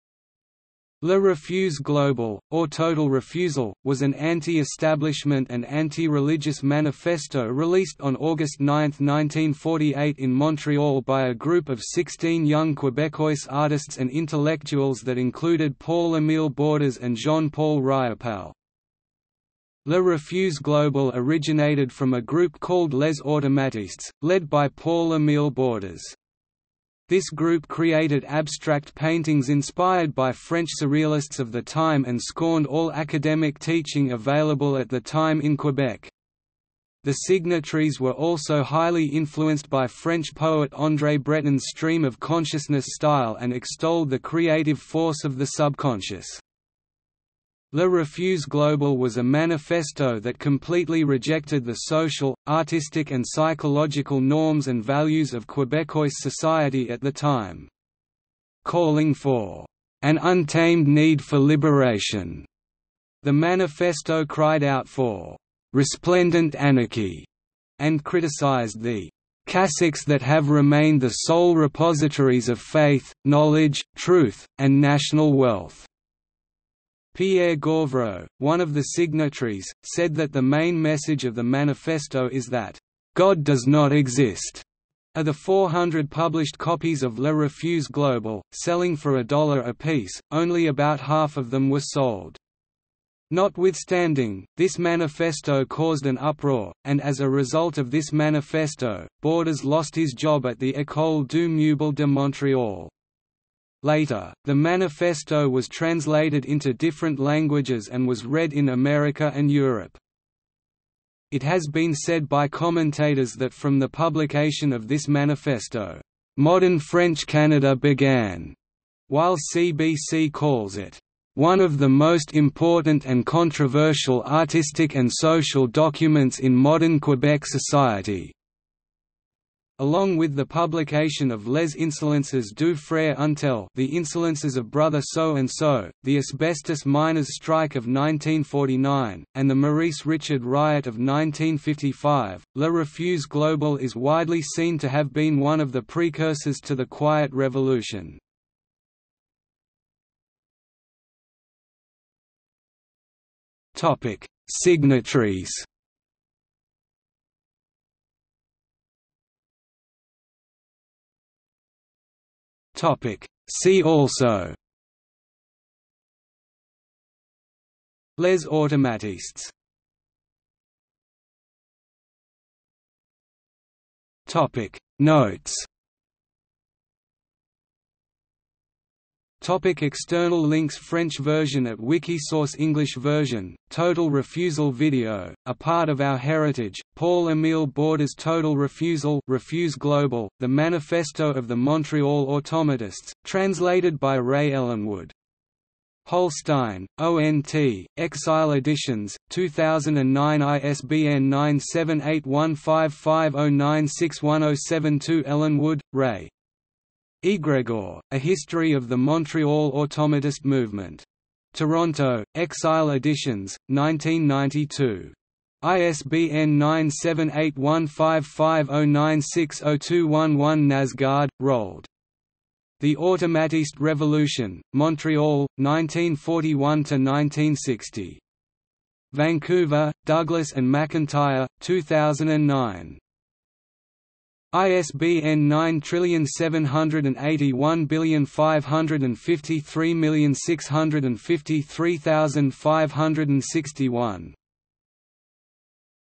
Refus Global | Wikipedia audio article
00:02:30 1 Signatories
00:02:39 2 See also
00:02:50 3 Notes
00:02:59 4 External links

- Socrates

SUMMARY
Le Refus global, or Total Refusal, was an anti-establishment and anti-religious manifesto released on August 9, 1948 in Montreal by a group of sixteen young Québécois artists and intellectuals that included Paul-Émile Borduas and Jean-Paul Riopelle.
Le Refus Global originated from a group called Les Automatistes, led by Paul-Émile Borduas. This group created abstract paintings inspired by French surrealists of the time and scorned all academic teaching available at the time in Quebec. The signatories were also highly influenced by French poet André Breton's stream-of-consciousness style and extolled the creative force of the subconscious.
Le Refus Global was a manifesto that completely rejected the social, artistic and psychological norms and values of Québécois society at the time. Calling for "an untamed need for liberation," the manifesto cried out for "resplendent anarchy" and criticized the "cassocks that have remained the sole repositories of faith, knowledge, truth, and national wealth." Pierre Gauvreau, one of the signatories, said that the main message of the manifesto is that "God does not exist." Of the 400 published copies of Le Refus Global, selling for a dollar apiece, only about half of them were sold. Notwithstanding, this manifesto caused an uproar, and as a result of this manifesto, Borduas lost his job at the École du meuble de Montréal. Later, the manifesto was translated into different languages and was read in America and Europe.
It has been said by commentators that from the publication of this manifesto, "modern French Canada began", while CBC calls it "one of the most important and controversial artistic and social documents in modern Quebec society". Along with the publication of Les insolences du Frère Untel (the Insolences of Brother So-and-so), the Asbestos miners' strike of 1949, and the Maurice Richard Riot of 1955, Le Refus Global is widely seen to have been one of the precursors to the Quiet Revolution.